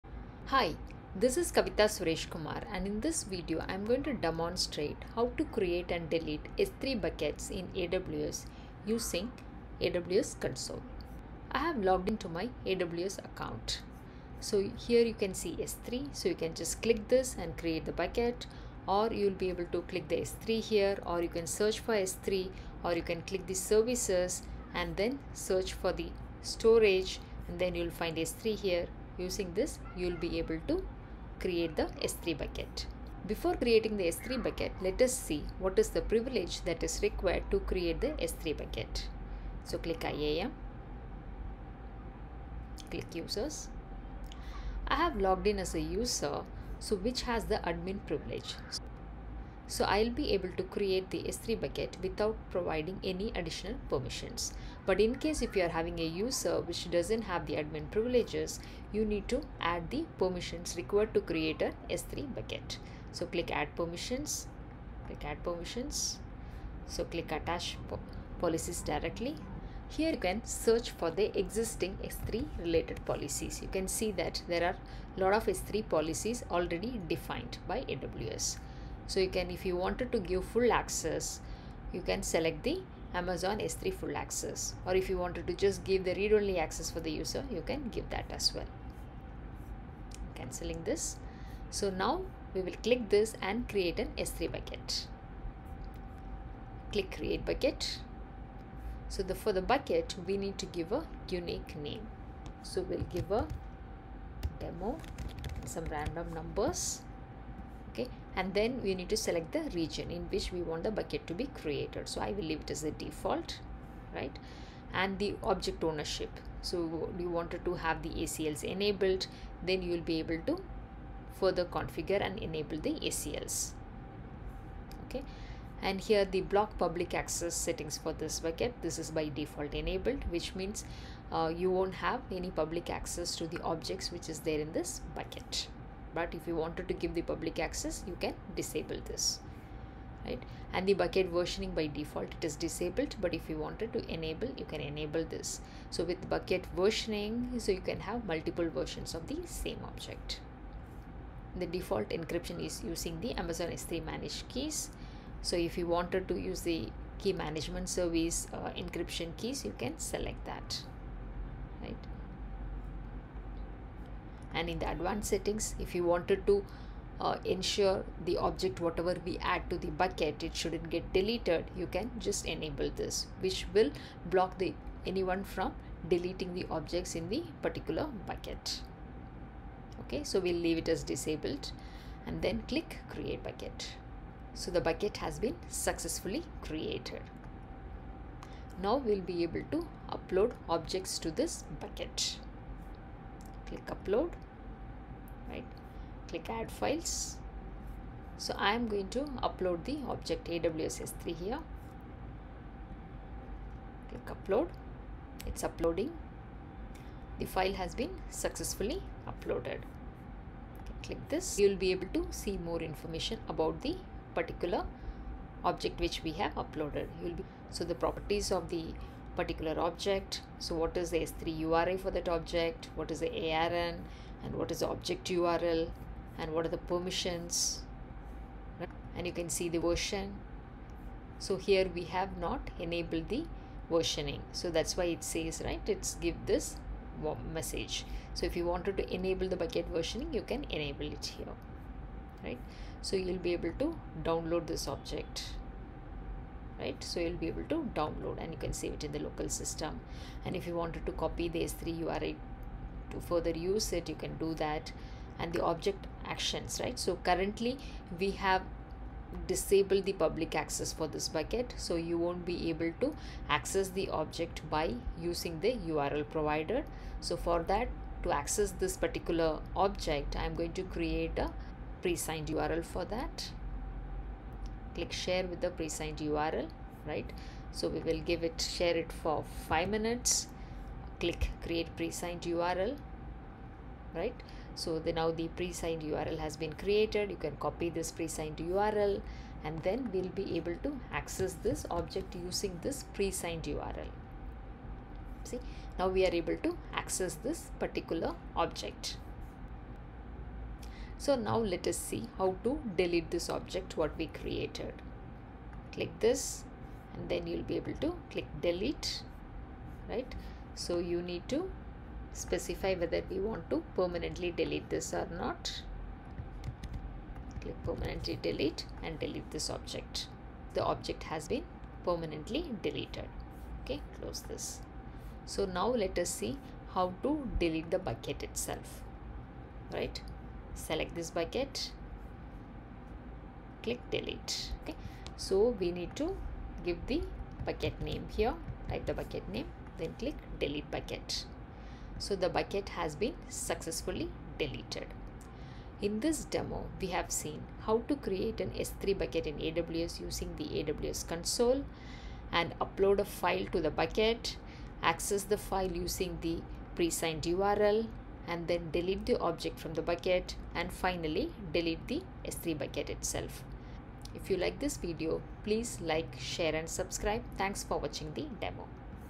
Creating and deleting S3 buckets in AWS using Console
In this video, you will learn how to create an S3 bucket in AWS using AWS Console, how to add objects to the bucket, access objects using pre-signed URL, delete objects from the bucket, and delete the S3 bucket.